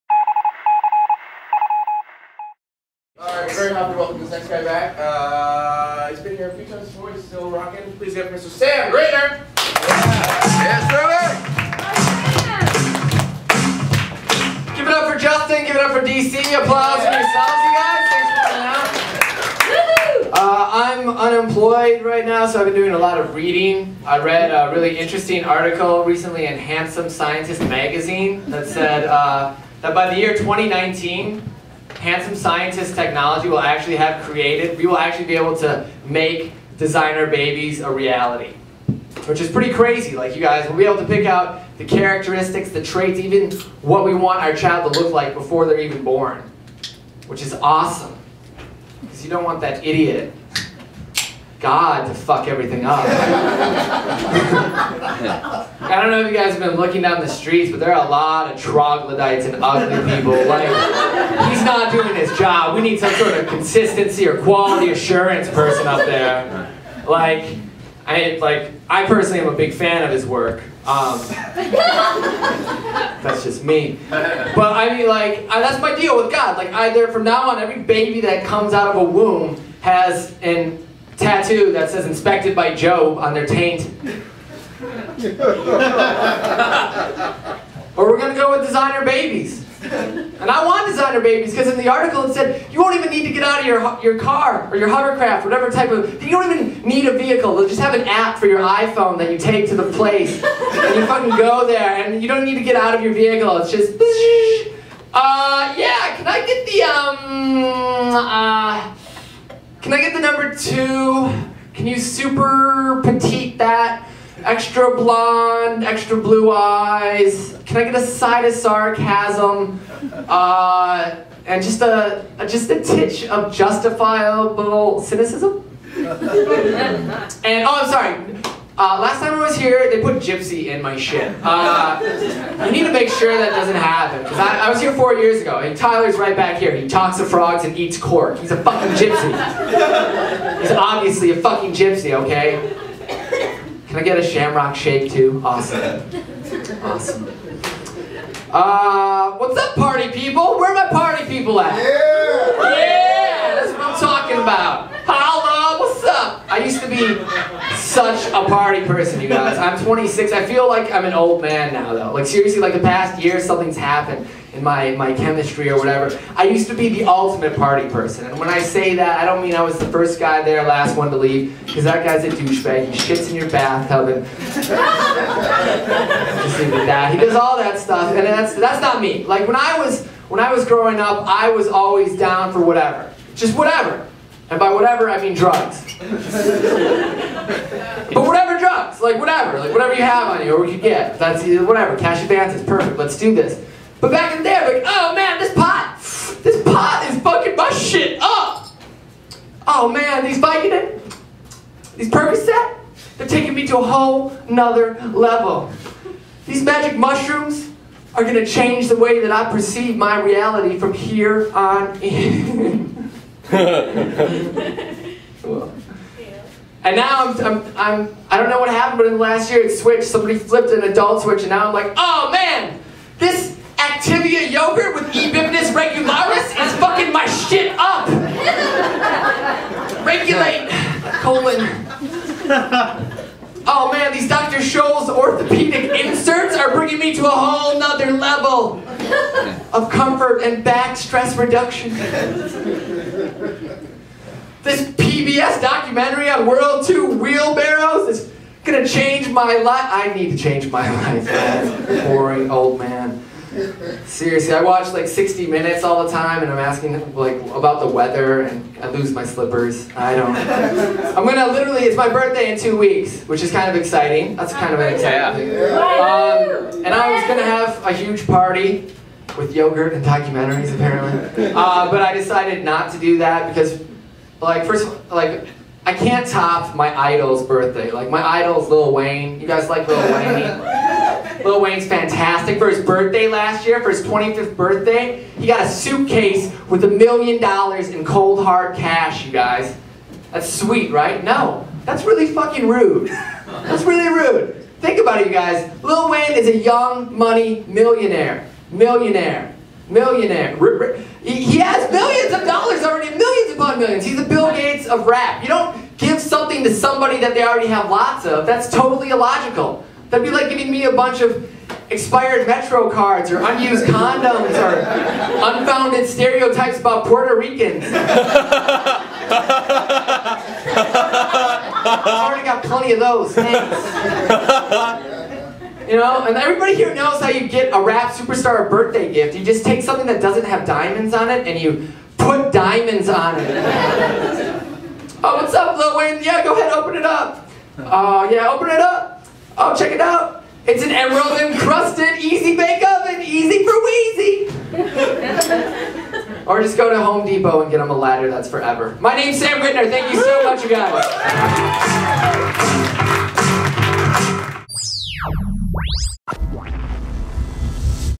School Night @ UCB April 2009
Sam performs stand-up at the infamous Upright Citizen's Brigade Theater in NYC. Thanks to Justin Purnell for hosting 'School Night!'